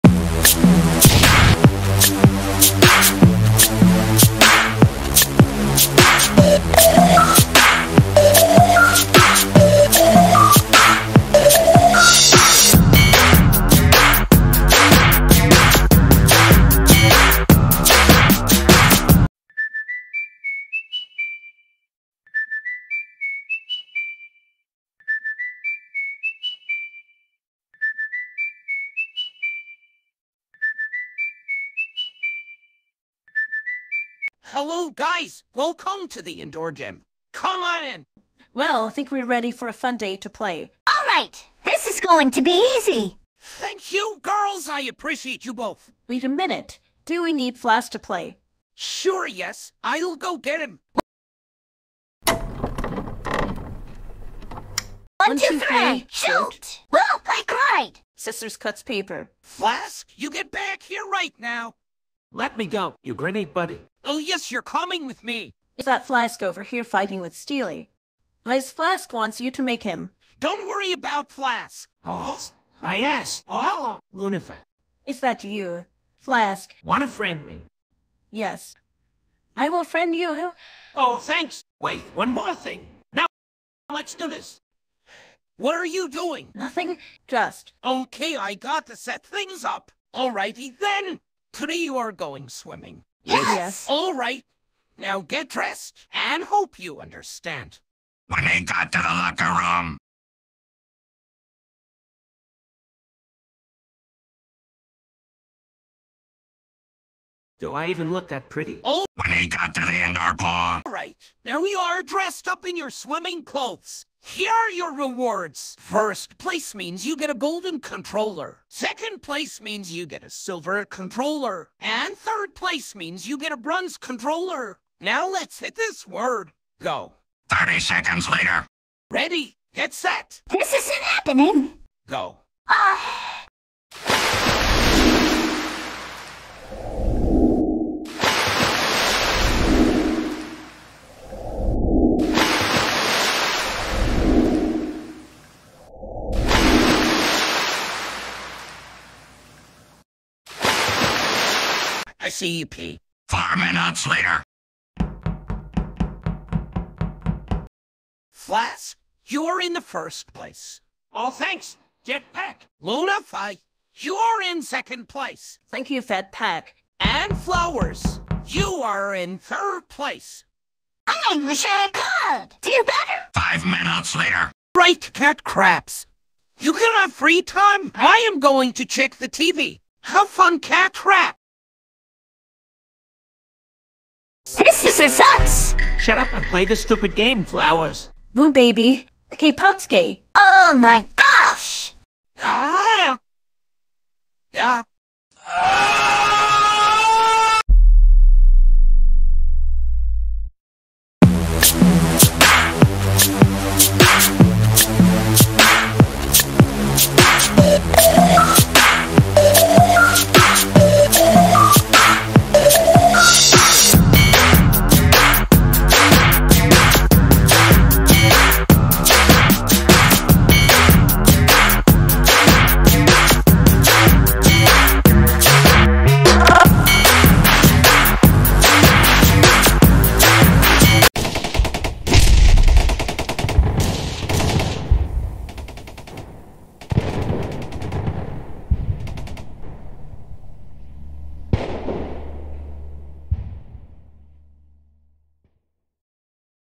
World Chibi Episode 3: SWIMMING GOOD
Lunnifa is a riding hood girl who is a fangirl. Flowers is a fairy who is 50% afraid and 50% brave enough. Steely is a U.S. soldier who trains them for swimming class. They were 2 girls named Lunnifa and Flowers who were so excited about the swimming class. A boy looks like a trainer named Steely's training class, but Flask is not understandable that he wants to do nothing outside. Steely makes the rules of how to swim. Think about the locker room and pool, Flask, Lunnifa, and Flowers were preparing to dress up with their swimming clothes or swimsuits to start swimming while Steely was watching them swimming better. But in the entertainment room, they were playing a type of "Among Us" game, product placement. And suddenly, Flowers was screaming because Lunnifa and Flask won except Flowers lost. And suddenly, Steely looks sleepy and normally painful while watching a scary movie or Flowers screaming when she lost a game.